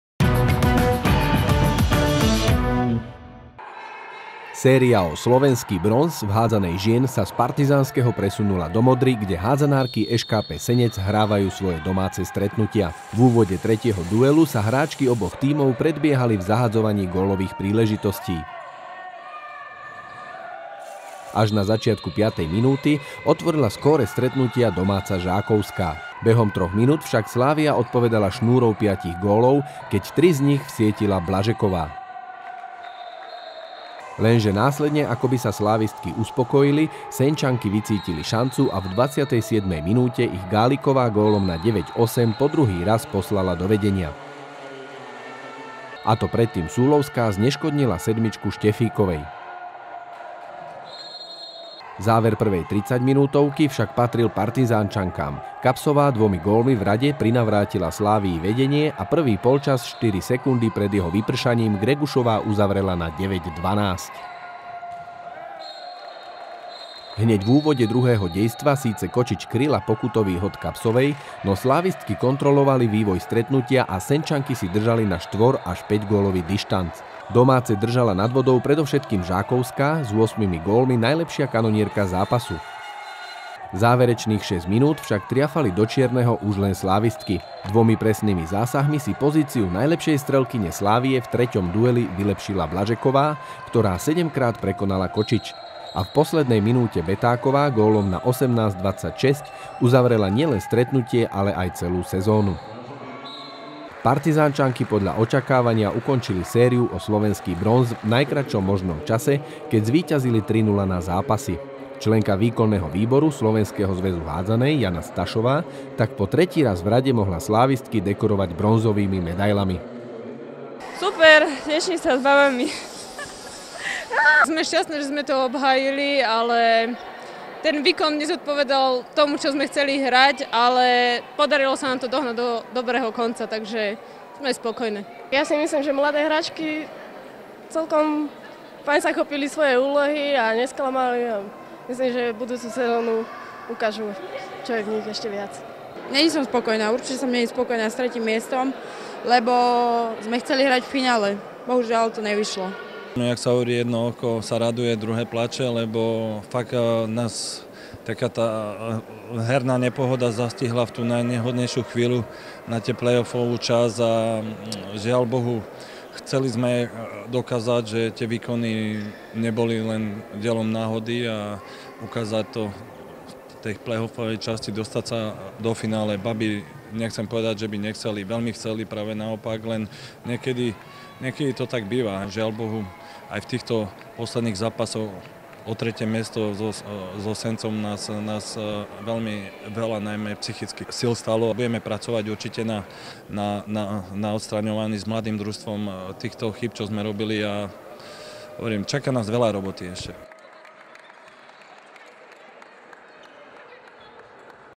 Slávistky opäť bronzové
Séria o slovenský bronz v hádzanej žien sa z Partizánskeho presunula do Modry, kde hádzanárky ŠKP Senec hrávajú svoje domáce stretnutia.